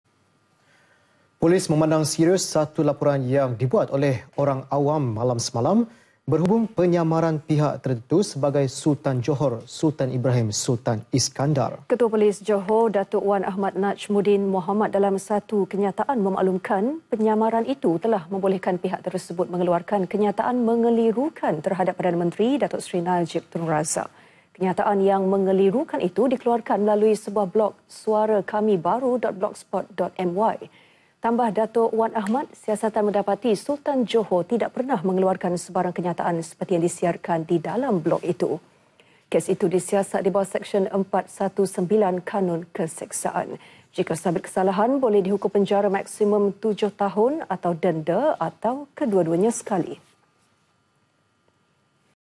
PENYAMARAN SEBAGAI SULTAN SELANGOR - POLIS MEMANDANG SERIUS [27 JULAI 2016]
Polis memandang serius  satu laporan  yang dibuat oleh orang awam MALAM SEMALAM  berhubung penyamaran  pihak tertentu sebagai Sultan  Johor, Sultan Ibrahim Sultan Iskandar.